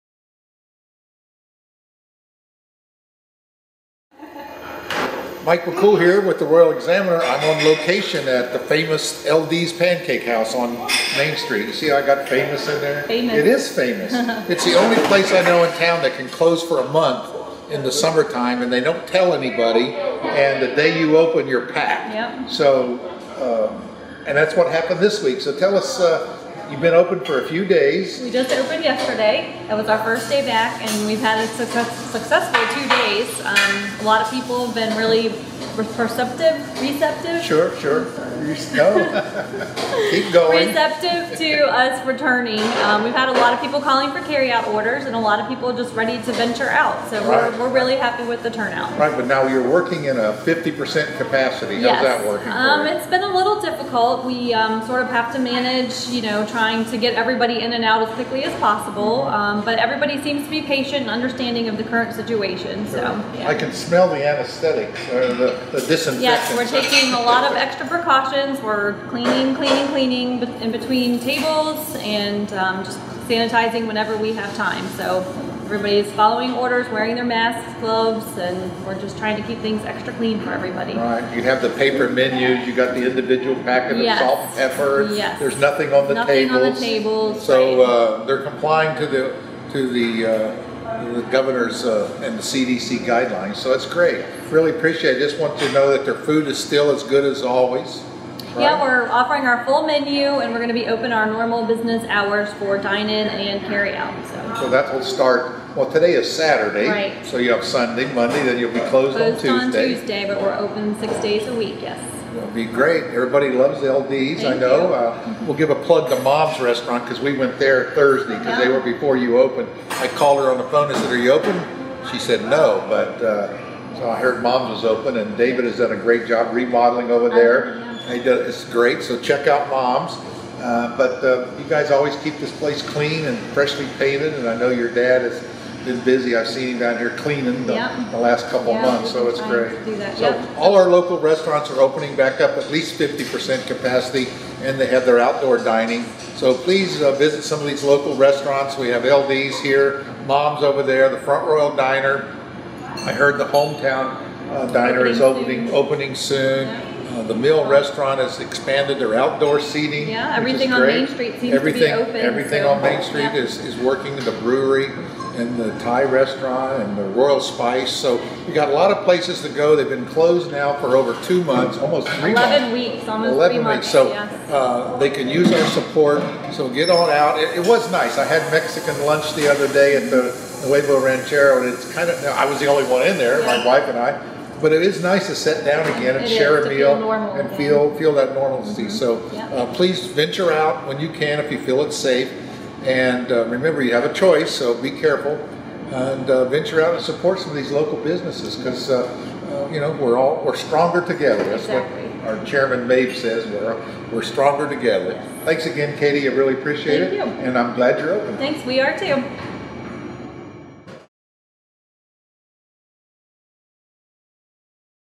Downtown Rebound: LDees Pancake House
On Saturday, June 13, 2020, our publisher Mike McCool, along with his grandson Connor Clark, went downtown to see how our local businesses were doing. First stop was LDees Pancake House. After lunch, they spoke with Katie Bradley about how things were going at LDees.